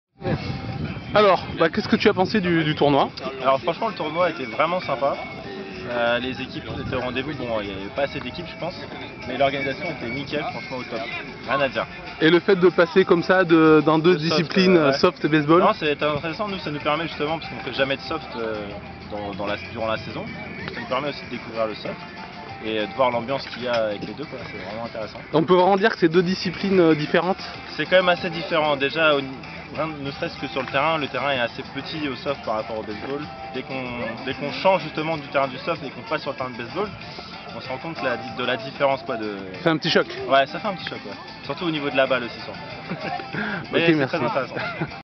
Patriots Paris - Tournoi "Les Patriots s'Emmêlent" 2016 itw #3
Interview d'un joueur des WEB'S Noisy Le Grand lors du tournoi "Les Patriots s'Emmêlent" 2016

PATRIOTS PARIS - Passionnés de Baseball & Softball depuis 1988